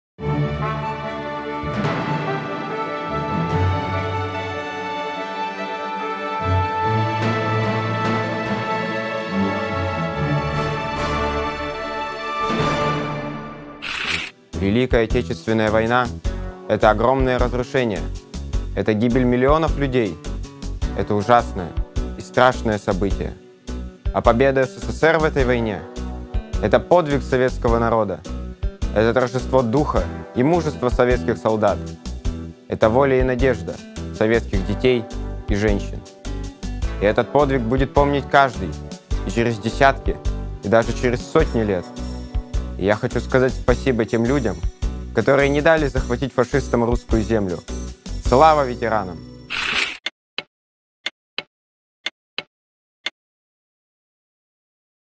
Мой День Победы. Константин Апицын
День Победы — это… Жители Нарьян-Мара рассказывают о том, что значит для них праздник 9 Мая, в проекте телеканала «Север», приуроченном к 70-летию Победы.